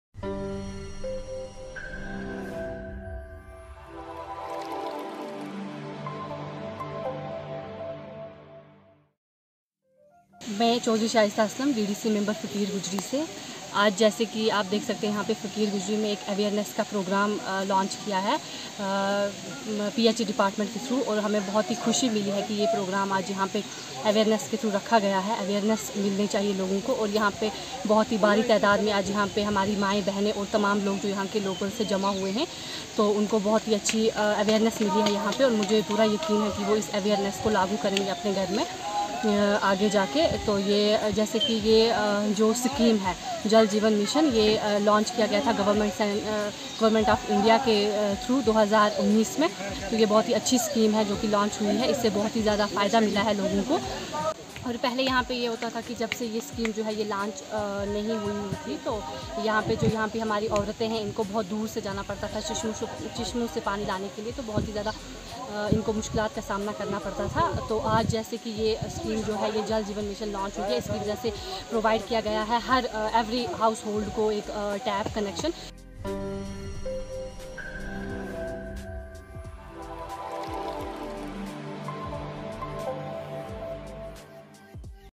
Jal Jeevan Mission ends our drudgery of fetching water - Ms. Shaista Chowdhary, J&K
SuccessStory from Jammu And Kashmir

"We have benefitted from this wonderful scheme JalJeevanMission launched by the Government of India. It ends our drudgery of fetching water in rain and snow. Now, every household of Faquir Gujri village in Srinagar gets clean tap water."- Ms. Shaista Chowdary, JammuAndKashmir.